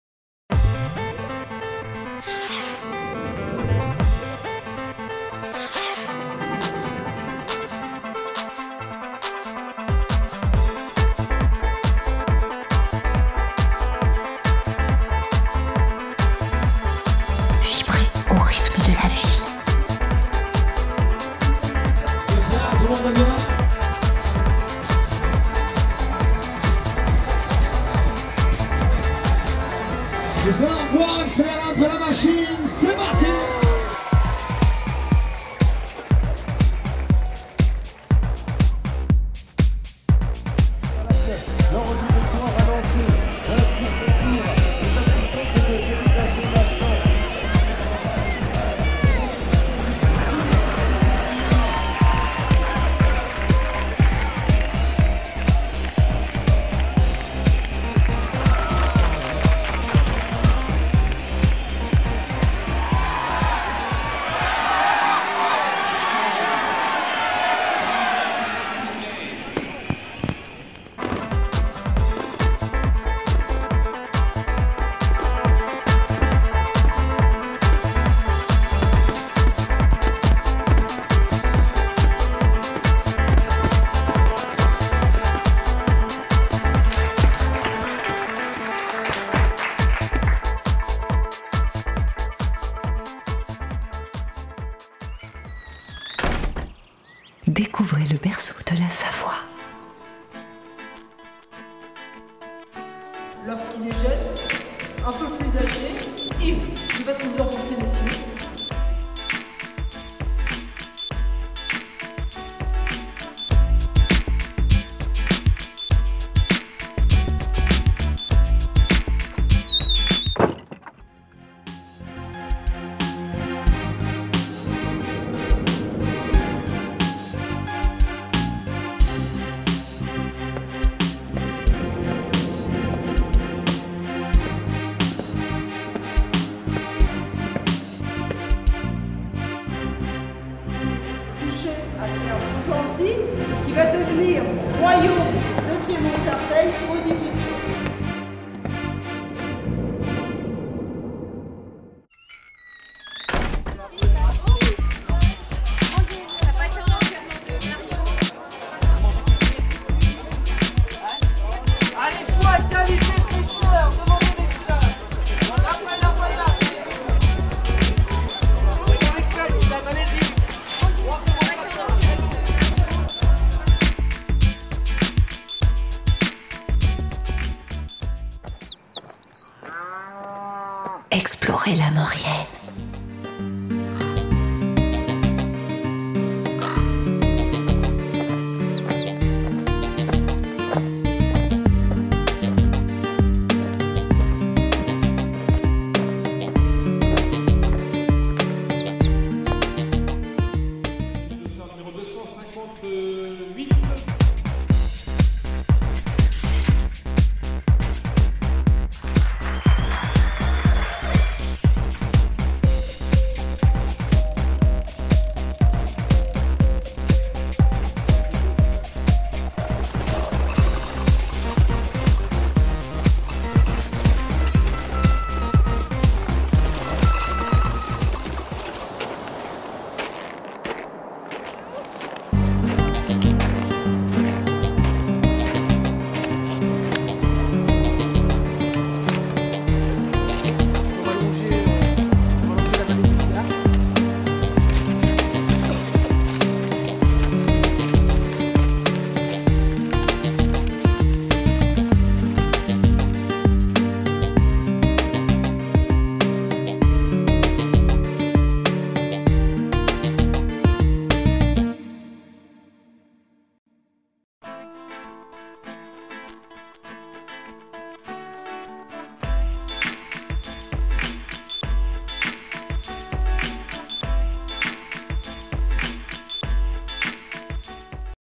Les villes étapes 2010 : visitez St Jean de Maurienne
Saint-Jean-de-Maurienne offre le riche patrimoine d'un Pays d'Art et d'Histoire : cathédrale et cloître gothique, crypte où l'on voit naître l'art roman, musée du costume, musée du Mont-Corbier (liqueur à base de plantes cueillies dans les montagnes environnantes) et, bien sûr, le musée de l'Opinel, le célèbre couteau inventé il y a plus d'un siècle à deux pas de la ville et qui a, depuis, fait le tour du monde.